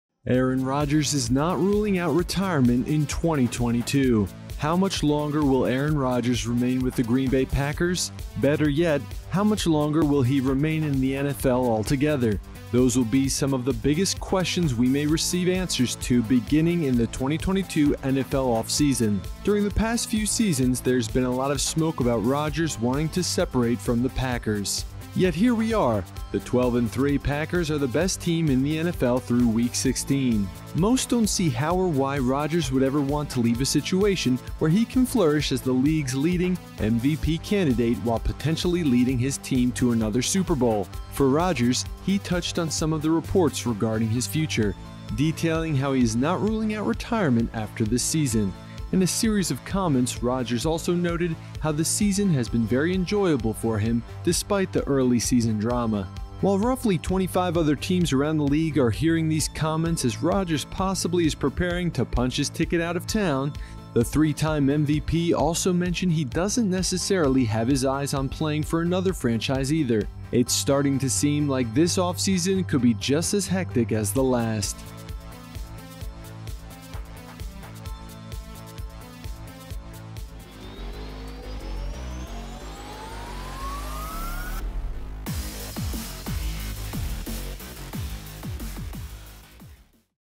Aaron Rodgers On Retirement In 2022
The BIGGEST NFL question this offseason... Will Rodgers retire?

How much longer will Aaron Rodgers remain with the Green Bay Packers?

Better yet, how much longer will AR12 remain in the NFL altogether?

During the past few seasons, there’s been a lot of smoke about Rodgers wanting to separate from the Packers. Yet here we are, the 12-3 Packers are the best team in the NFL through Week 16.

Most don’t see how or why Rodgers would ever want to leave a situation where he can flourish as the league’s leading MVP candidate while potentially leading his team to another Super Bowl.